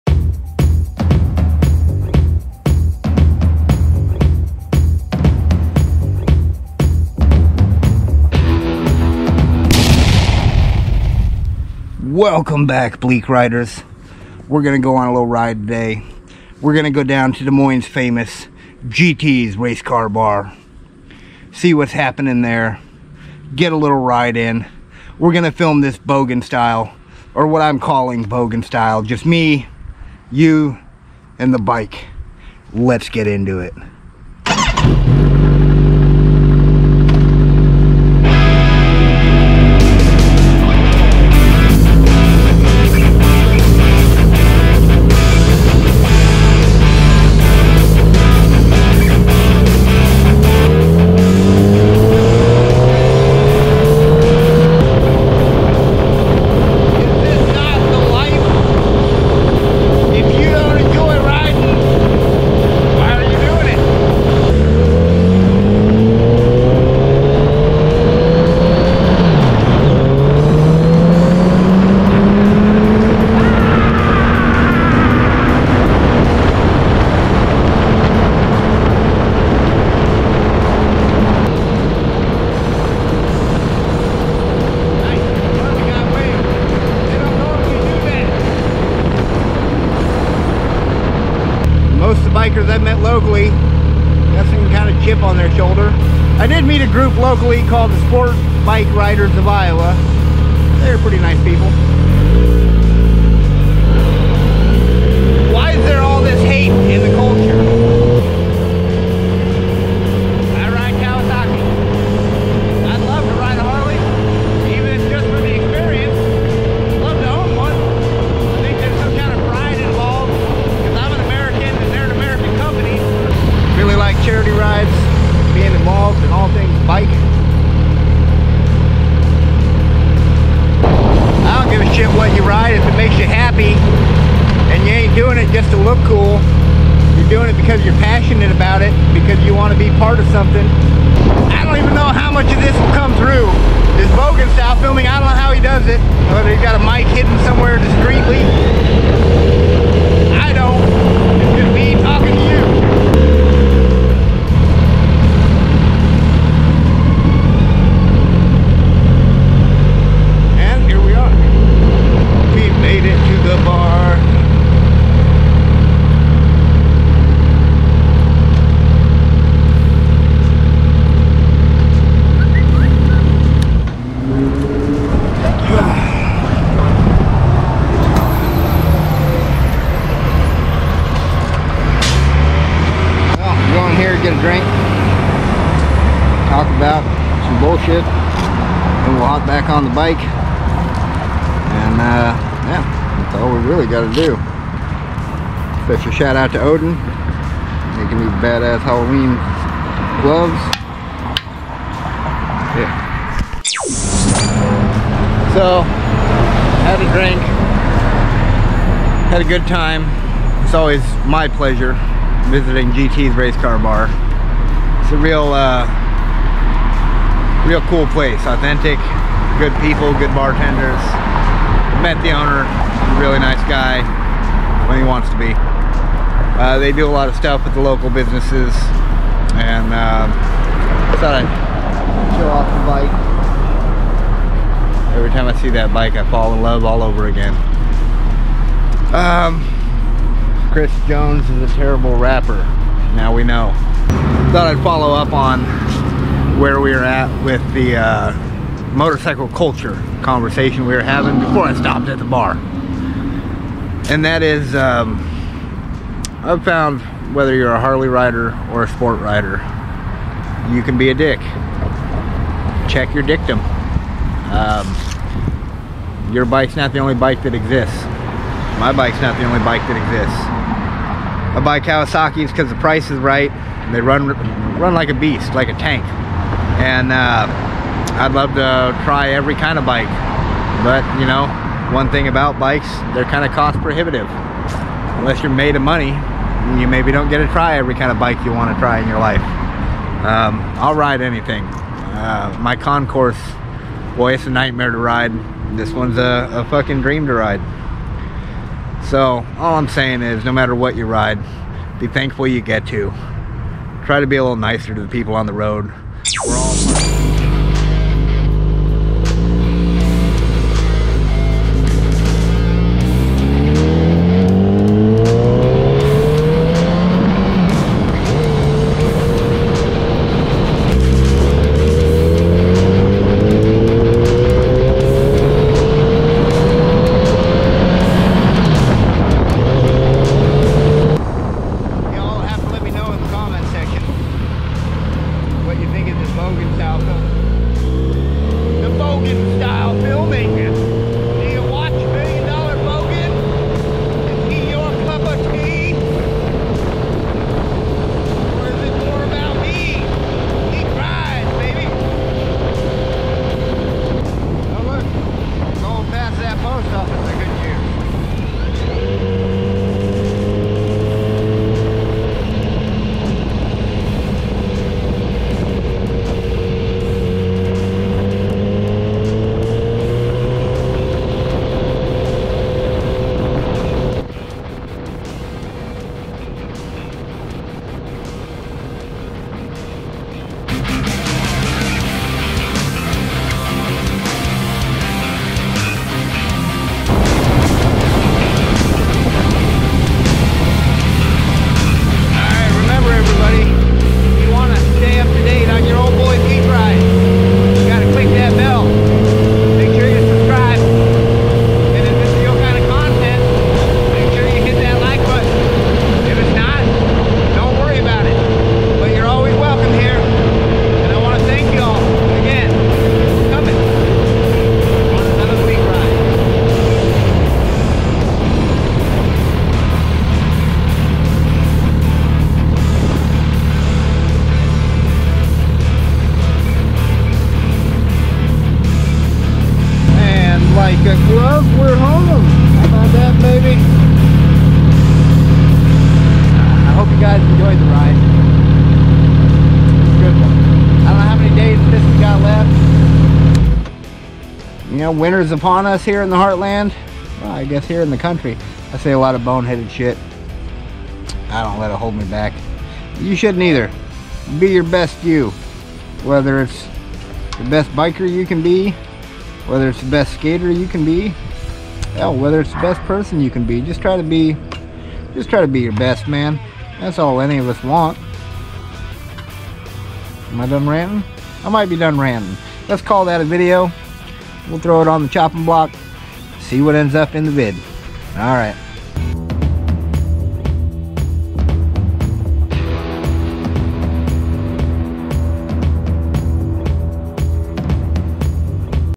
Guerrilla Bogan style filming. Also a rant about A-holes in bike culture.
A short ride to a exception to the rule of bike bars. some real gonzo reporting. A rant about the Brand purists. Also bike culture. This one may upset of some viewers.